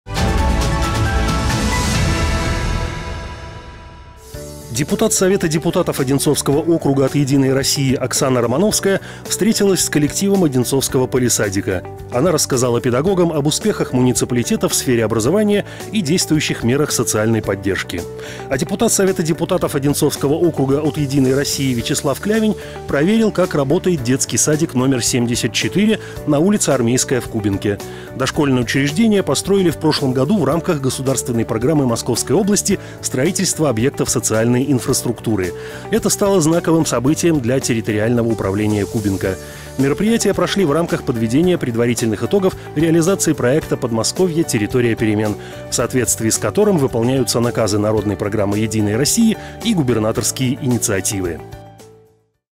Дошкольные учреждения округа
Депутат Совета депутатов Одинцовского округа от «Единой России» Оксана Романовская встретилась с коллективом одинцовского «Палисадика». Она рассказала педагогам об успехах муниципалитета в сфере образования и действующих мерах социальной поддержки. А депутат Совета депутатов Одинцовского округа от «Единой России» Вячеслав Клявинь проверил, как работает детский садик №74 на улице Армейская в Кубинке. Дошкольное учреждение построили в прошлом году в рамках государственной программы Московской области «Строительство объектов социальной инфраструктуры». Это стало знаковым событием для территориального управления Кубинка. Мероприятия прошли в рамках подведения предварительных итогов реализации проекта «Подмосковье – территория перемен», в соответствии с которым выполняются наказы Народной программы «Единой России» и губернаторские инициативы.